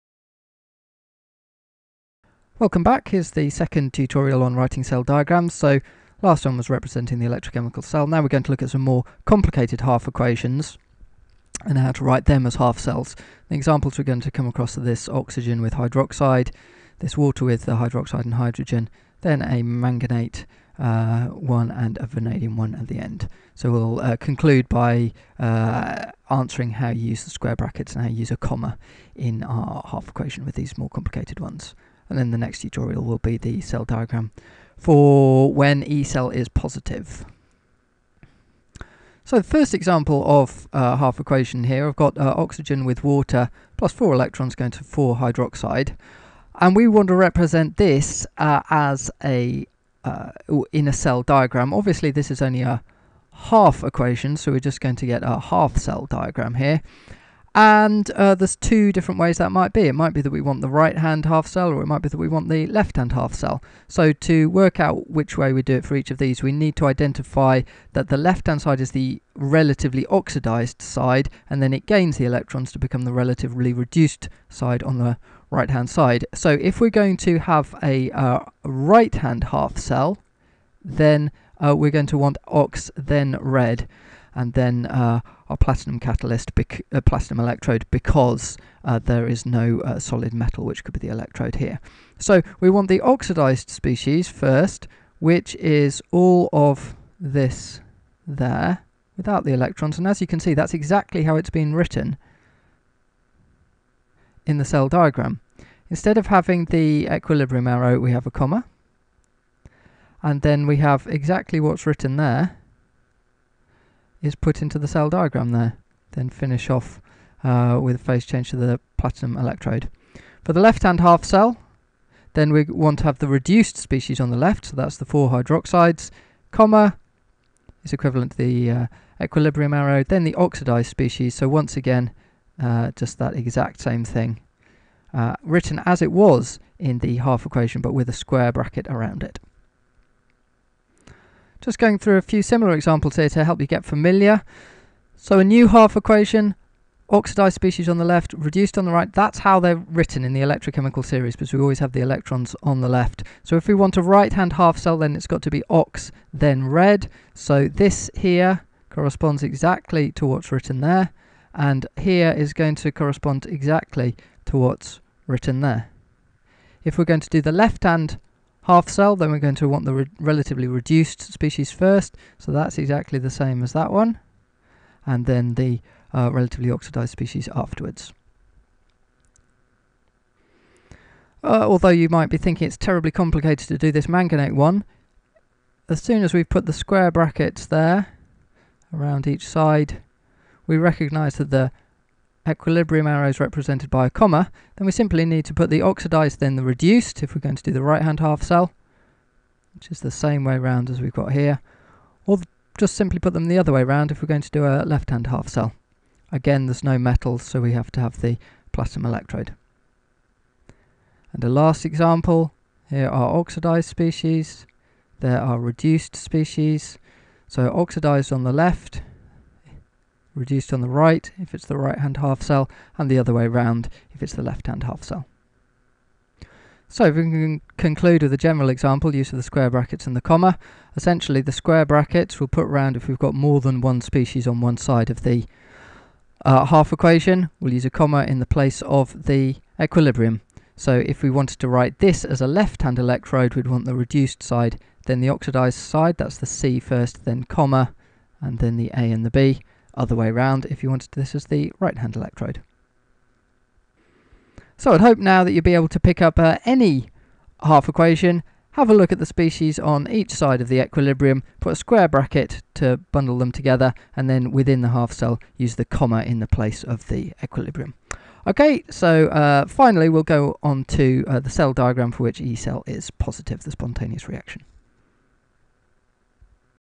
Electrochemistry - The Cell Diagram (2)
2. More complicated half-equations written as half-cells
a. Examples
i. O2 + 2H2O + 4e--⇌ 4OH--
ii. 2H2O + 2e-- ⇌ 2OH-- + H2
iii. MnO42- + 2H2O + 2e- ⇌ MnO2 + 4OH-
iv. VO2+ + 2H+ + e- ⇌ V3+ + H2O
b. Conclusions
i. use of [  ]
ii. use of ,